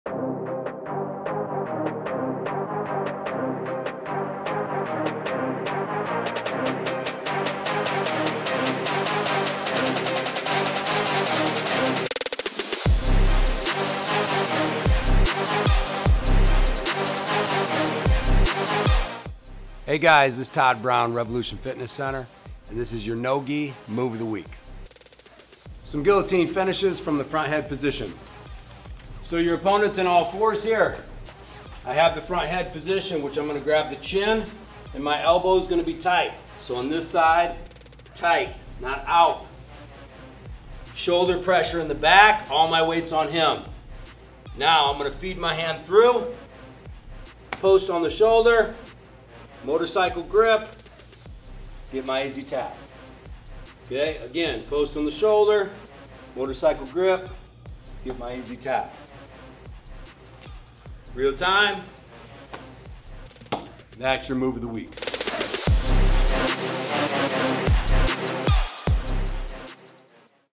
MOTW | No-Gi | Guillotine Finishes from Front Head Position
Join Todd Brown as he demonstrates Guillotine Finishes from Front Head Position in this week's No-Gi Move of the Week video. Master this submission hold and add it to your arsenal of attacks.